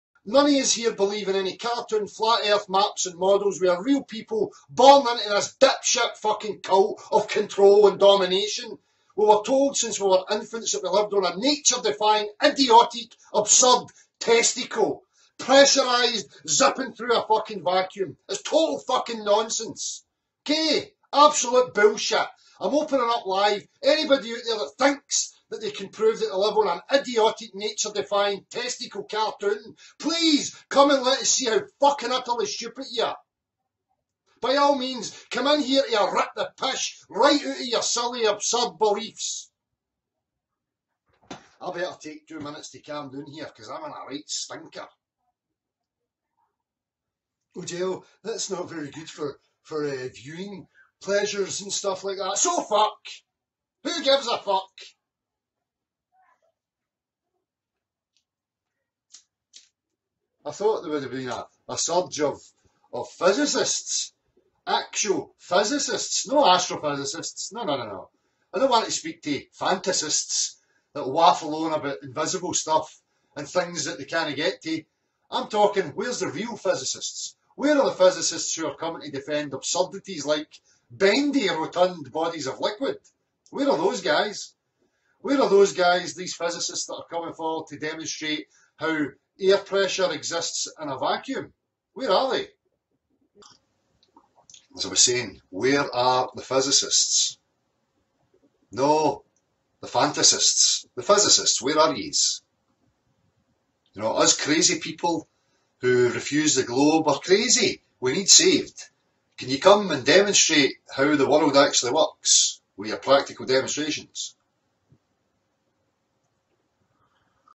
Globe Earth Is An Embarrassment (Beyond The Imaginary Curve)
This video is the same as my last upload except I added memes onto it. Hope you enjoy it. The globe is a joke.